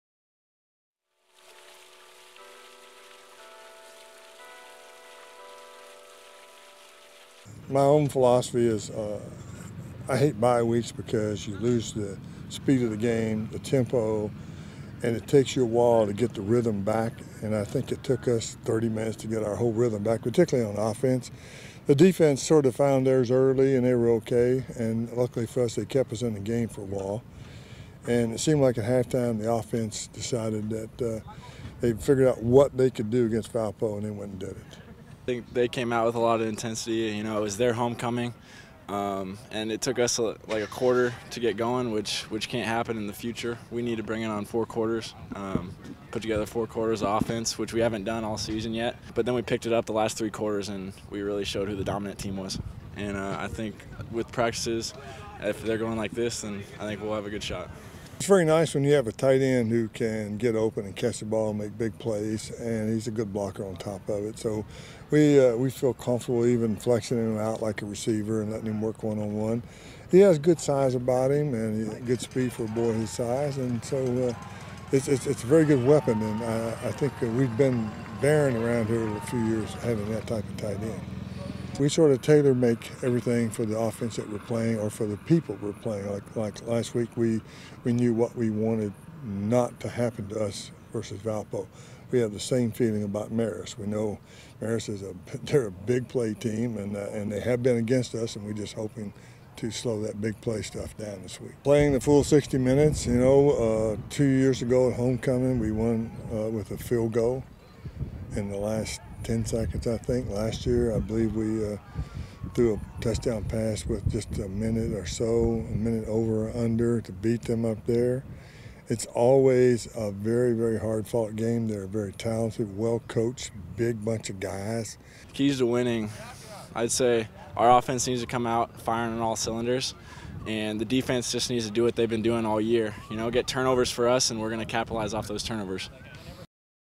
Marist Preview
Coach Lindsey and Ross Dwelley reflect on last week's 38-6 win at Valpo and preview Saturday's home game against Marist.